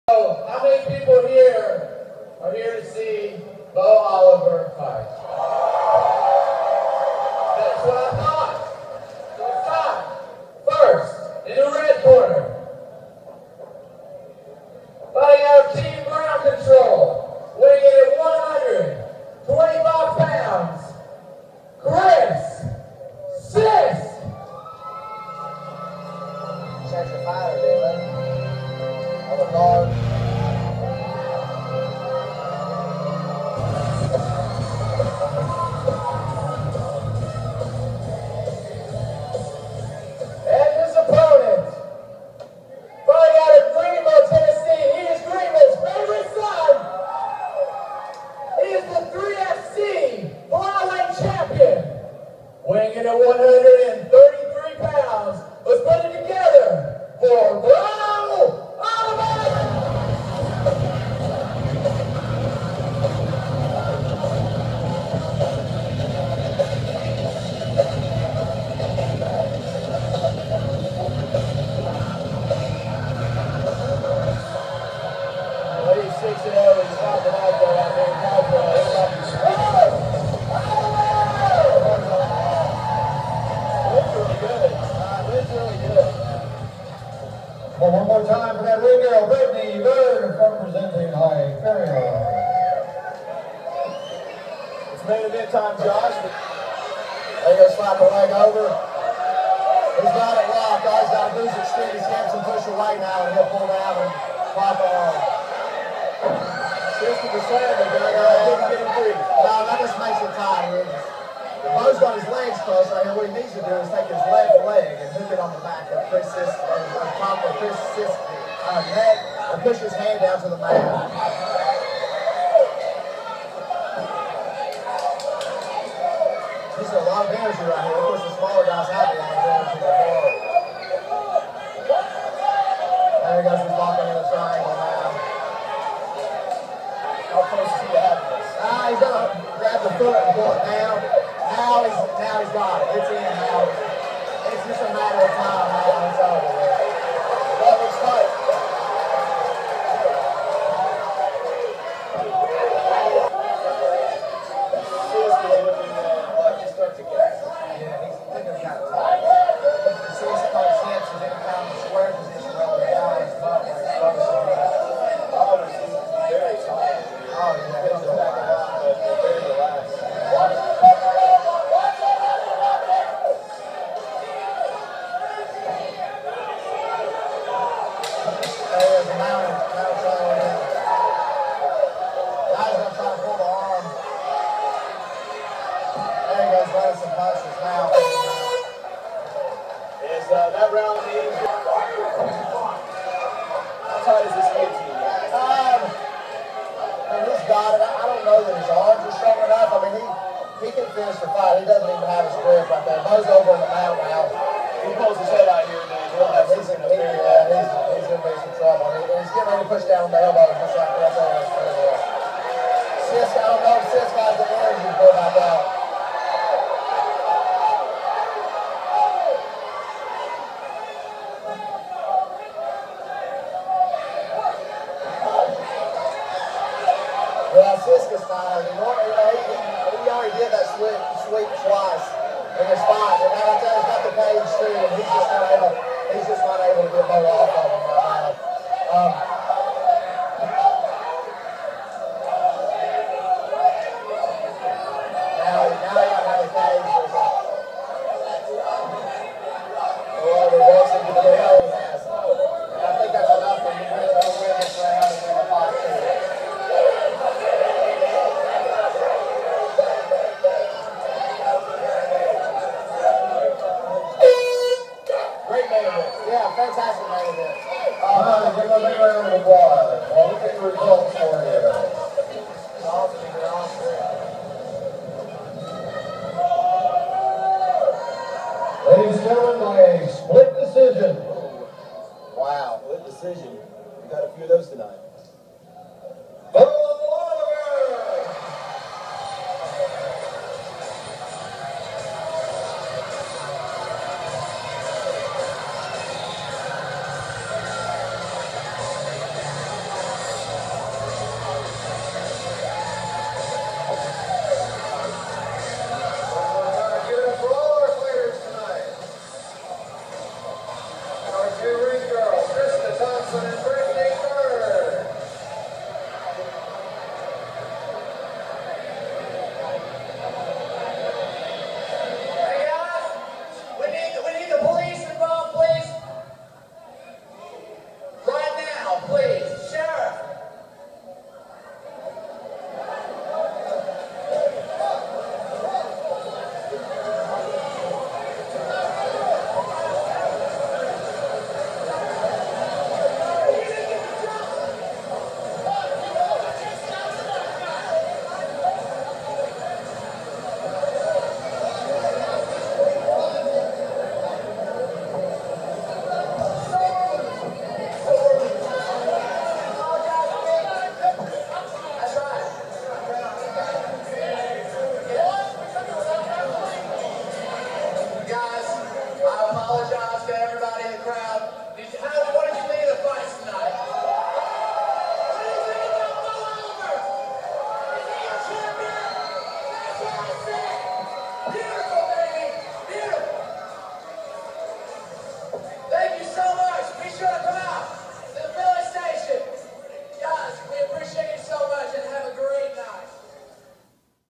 Bo Oliver MMA Uprising Highlight
Highlights of the main event of the first ever MIxed Martial Arts show in Greeneville TN at Hal Henard Arenwith a bit of unsportsman-like behavior at the end.